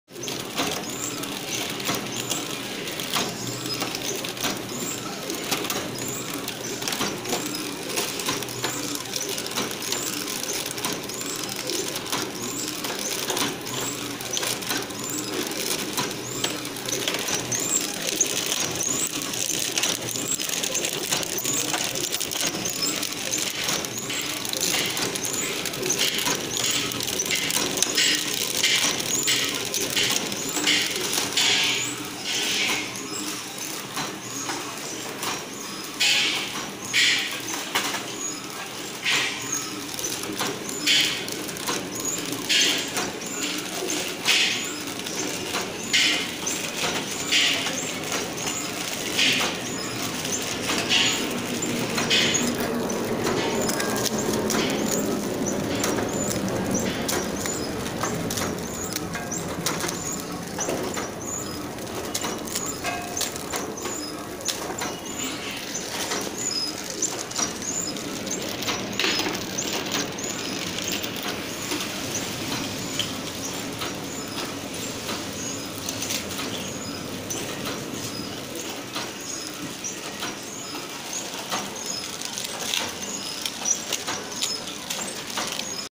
Chiki Packing,Mini Flow Wrap Machine(Price: 1.9L)PLC Base Economical Model - ECO
Mini(table top) Flow Wrap Machine. PLC Base Economical Mode - ECO

PRICE: 1,90,000 INR.

# Most Innovative Features Compare With Others: Machine will work on different pouch sizes (i.e.. min. pouch length 60 mm to max. 200 mm) without any gear changes or any mechanical adjustment. You just enter your required pouch length into the PLC controler & it will change automatically

Product to be pack -
Length :
Min. - 1"(25 mm)
Max. - 6"(150 mm)

Width :
Min. - 0.5"(13 mm)
Max. - 2.5"(65 mm)

Height : up to 1.5"(38 mm)

OR
Foil width is not more than 200 mm.